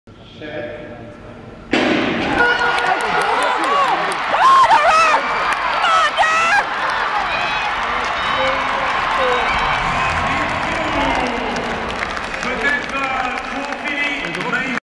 European Indoor 2011 60mh Final.MOV
Rank   Athlete Nation Result
1    NYTRA, Carolin   GER 7.80  EL
2    OFILI, Tiffany   GBR 7.80  NR =EL
3    VUKICEVIC, Christina   NOR 7.83  NR
4    O'ROURKE, Derval   IRL 7.96  SB
5    TALAI, Alina   BLR 7.98
6    ANTONOVA, Aleksandra   RUS 8.00
7    SKROBÁKOVÁ, Lucie   CZE 8.10
8    GOMIS, Sandra   FRA 8.11